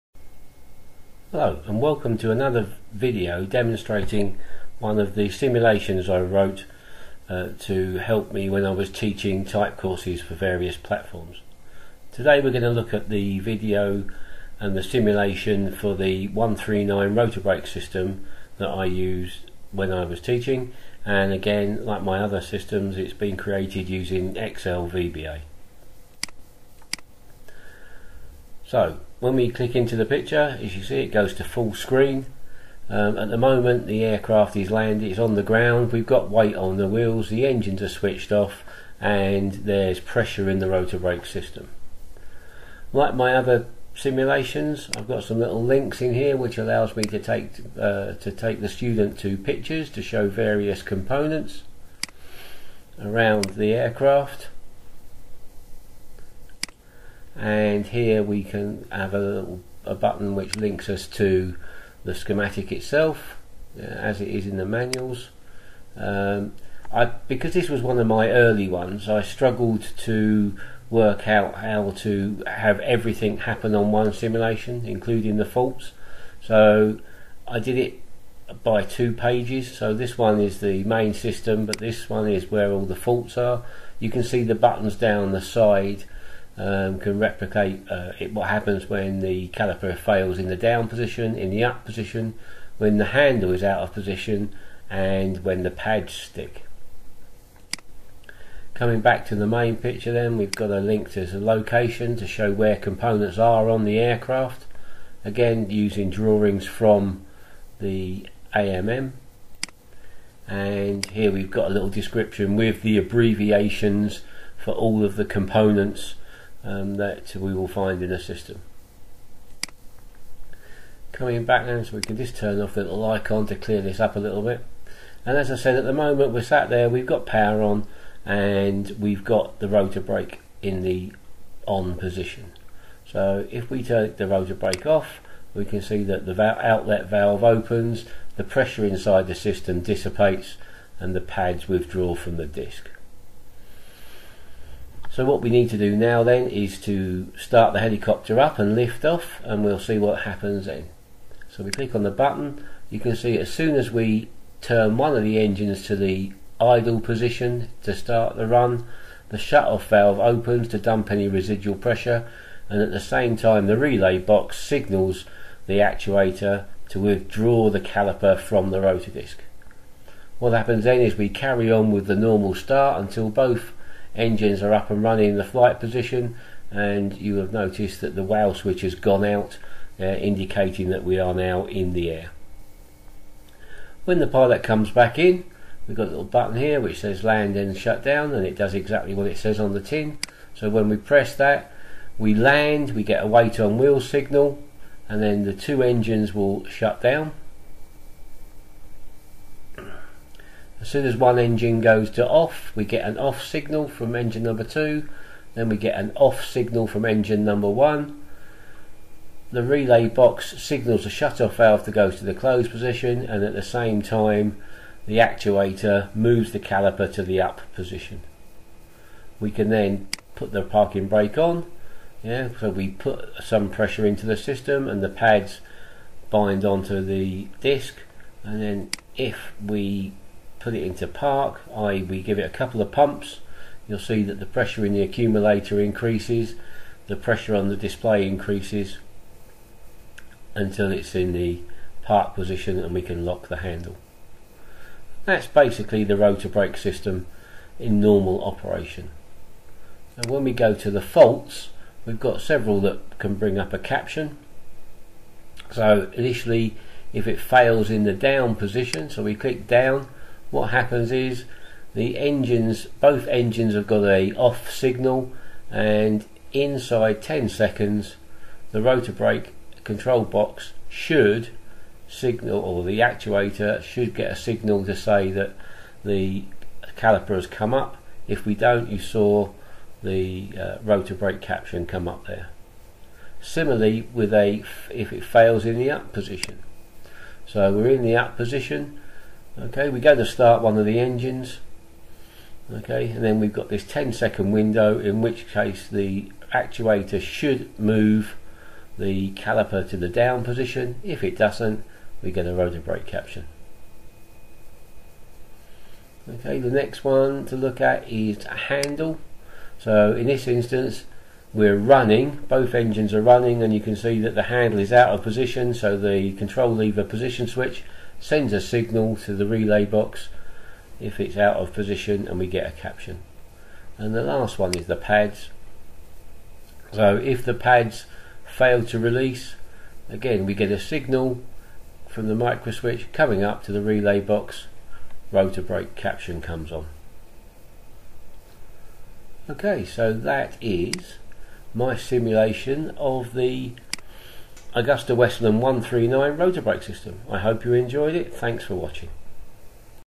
AW139 Rotor Brake System
This video shows my simulation of the AW139 rotor brake system. It was created using Excel VBA, and shows normal operation and faults.

Please check out my survey on costs for selling the simulations